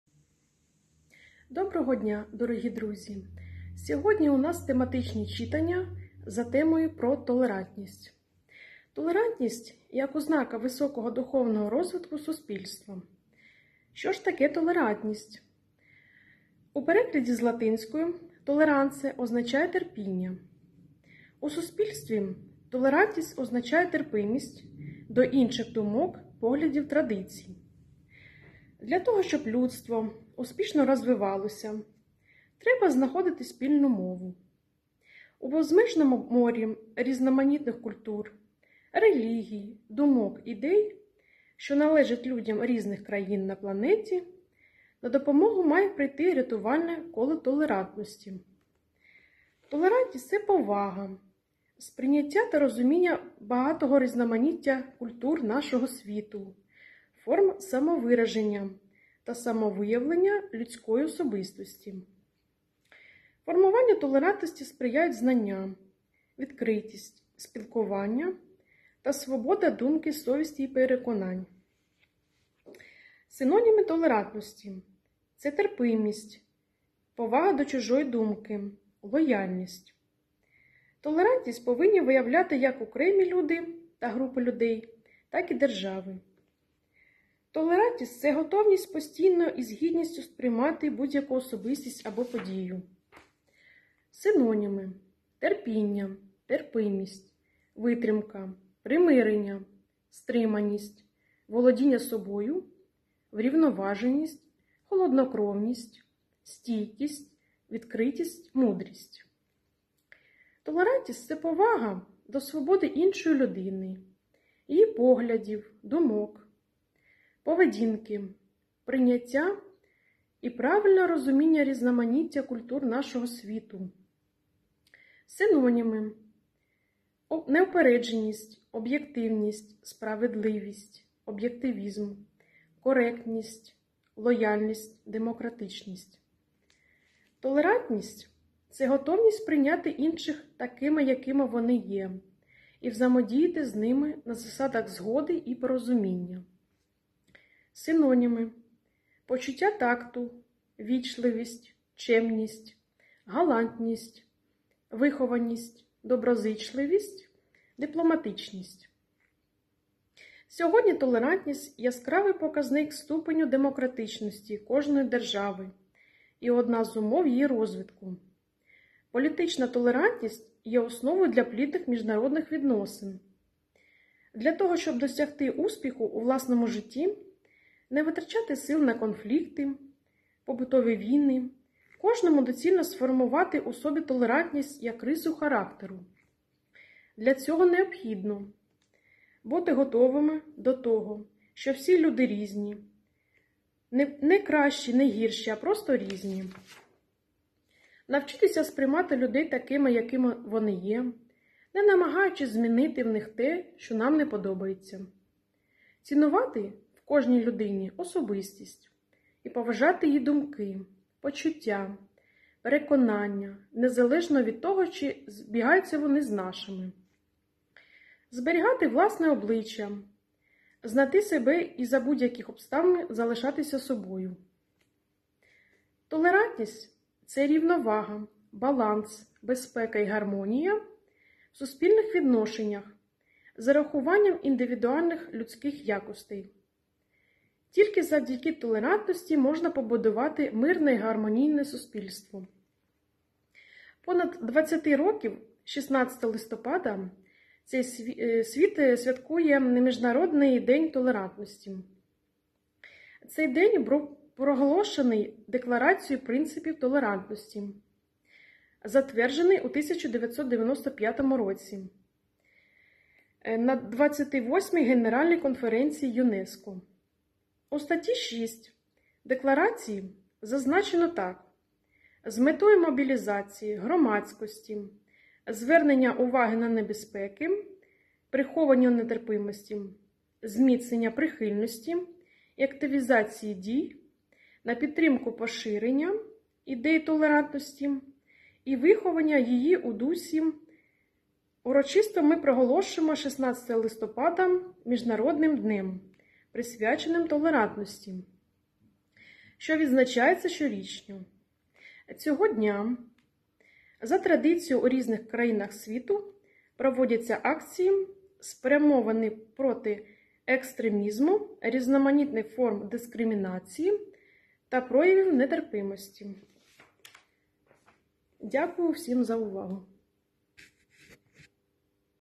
"Про толерантність". Тематичні читання
Про поняття толерантності  з книжок.
БСЧ імені Олеся Гончара у соцмережах:
фейсбук: Бібліотека сімейного читання ім. Олеся Гончара
твітер: БСЧ ім. Олеся Гончара (@Zw5G4HbT3Xj5504) | Twitter